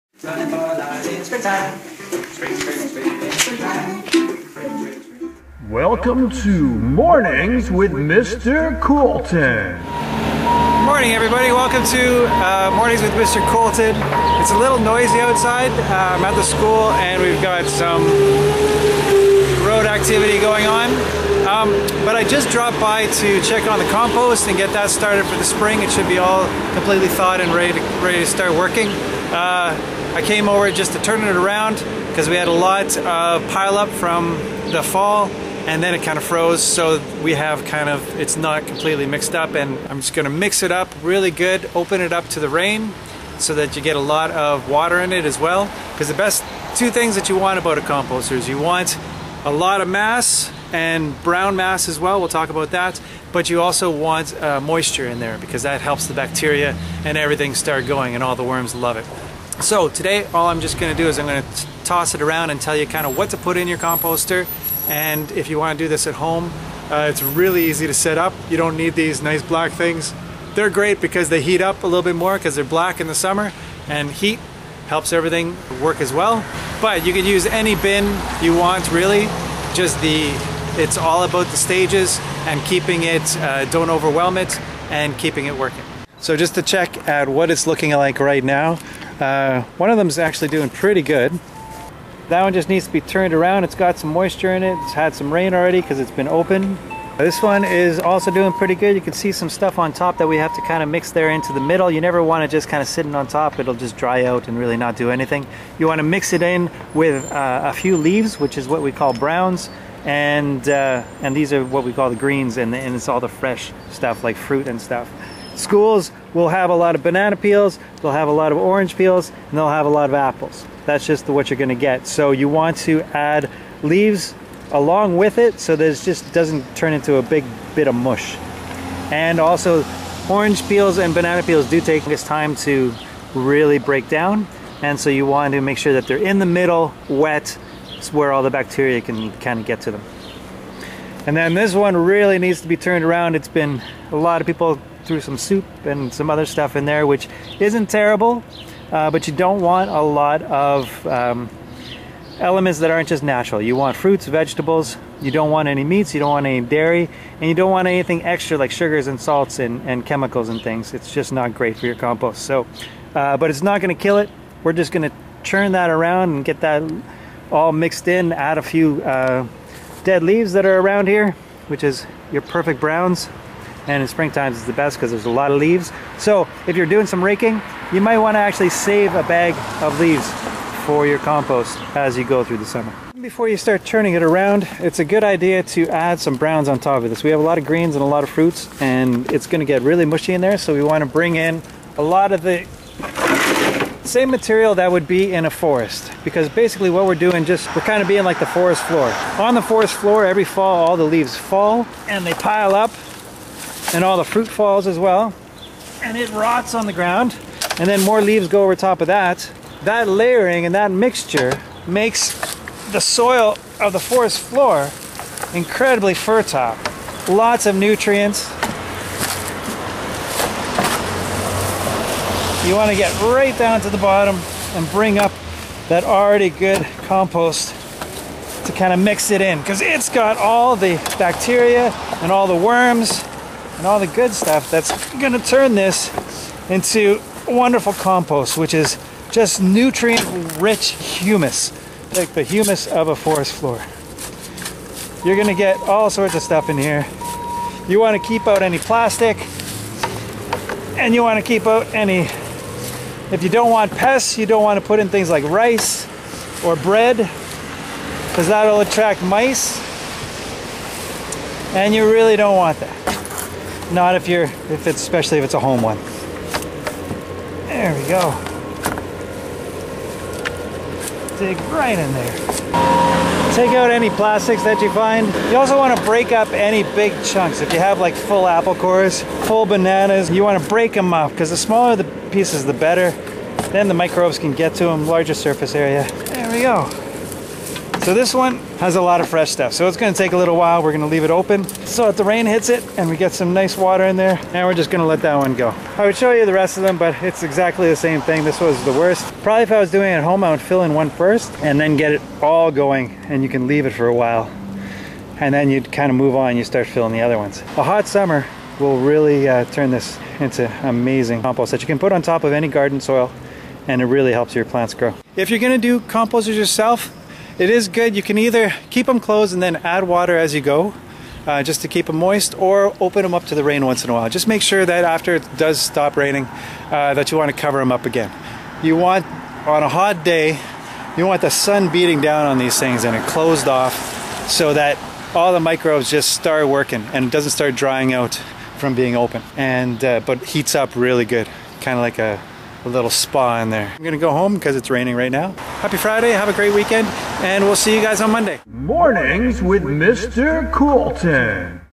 Mr Kolton in the Morning-Episode 27 -Composter
In this episode Mr Kolton, gets the school composters started up for the spring.